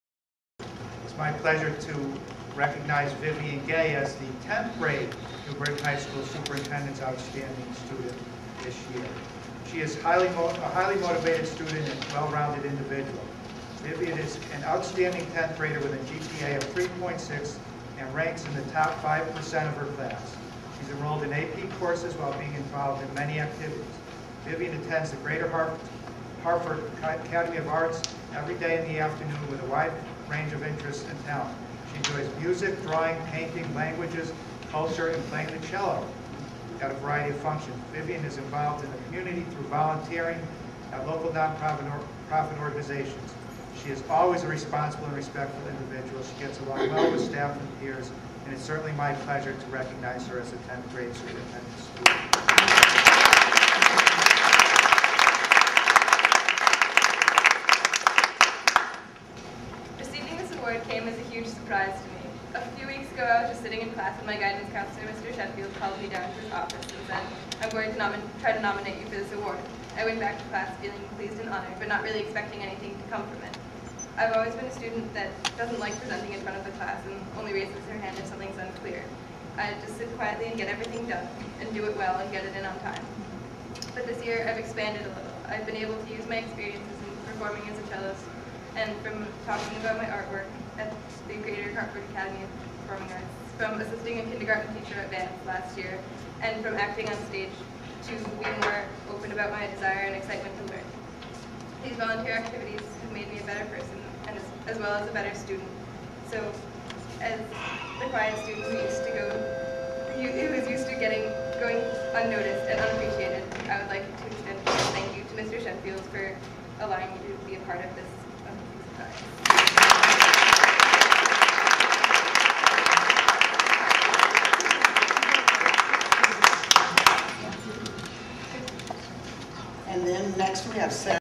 Vivian's Outstanding Student Award
Viv recieved teh Superintendent's Outstanding Student of the year award for the New Britain High sophmore class.